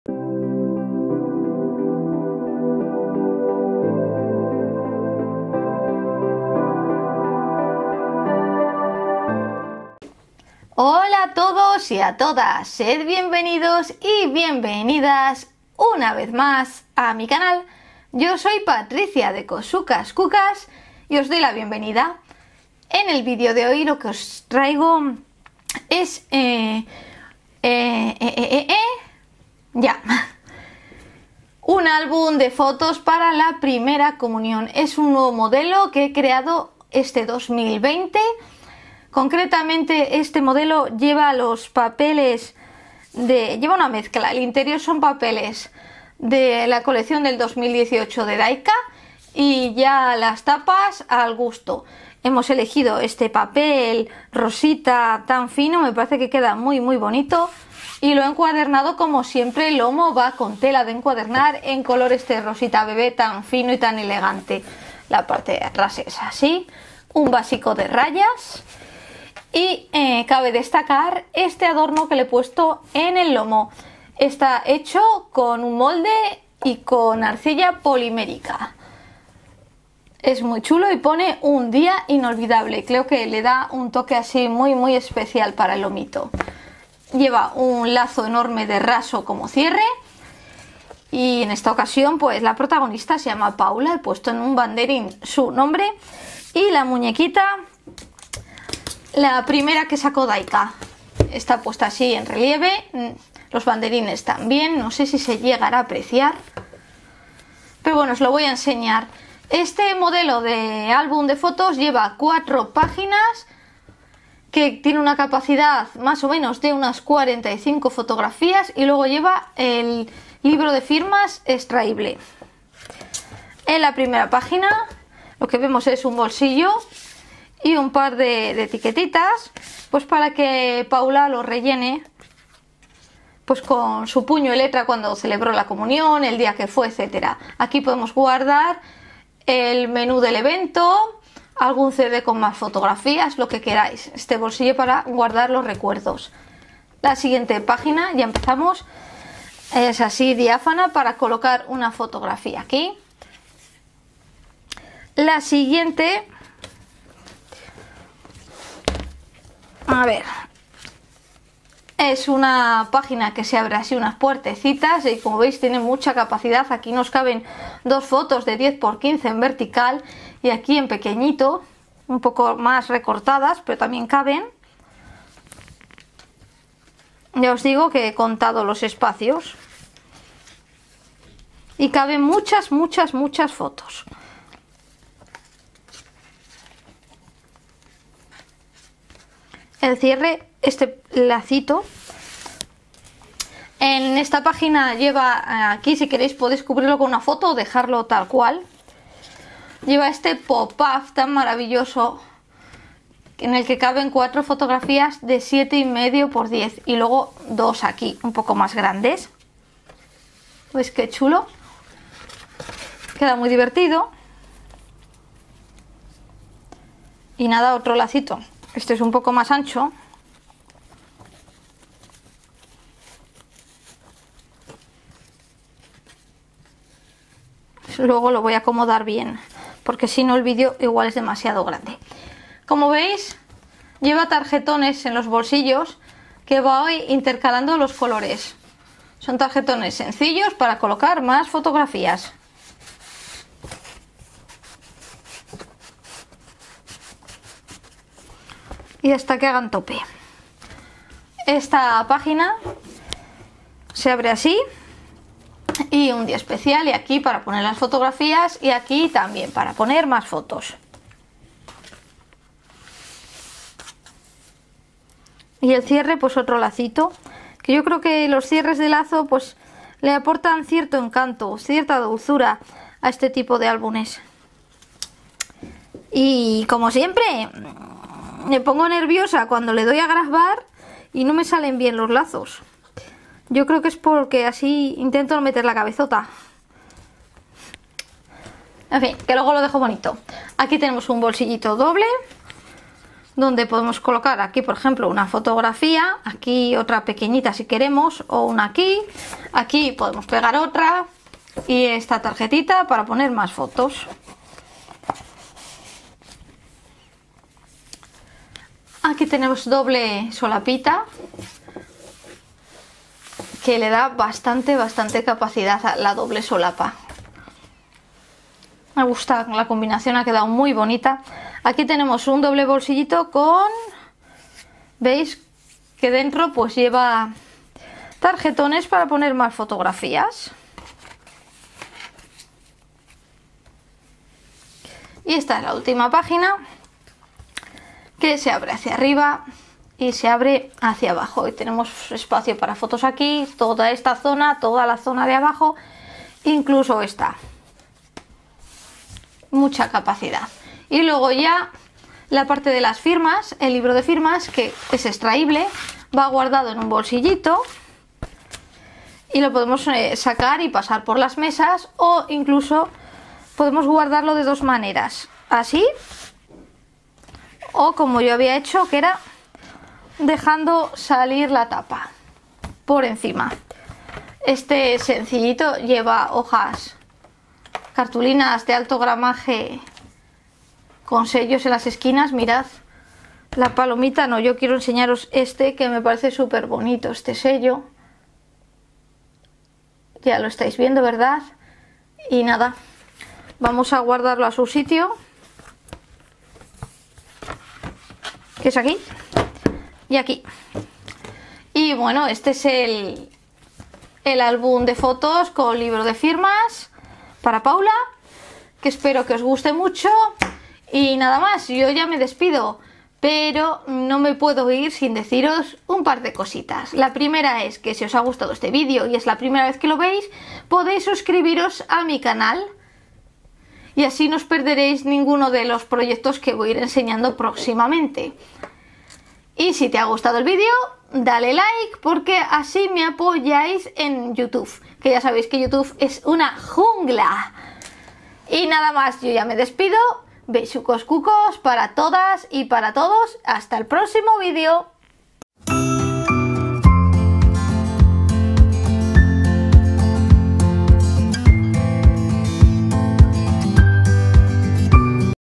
COMUNIONES 2020 álbum y libro de firmas dayka
¡Hola! hoy os enseño otra versión del álbum de comunión con librodefirmas y papeles de dayka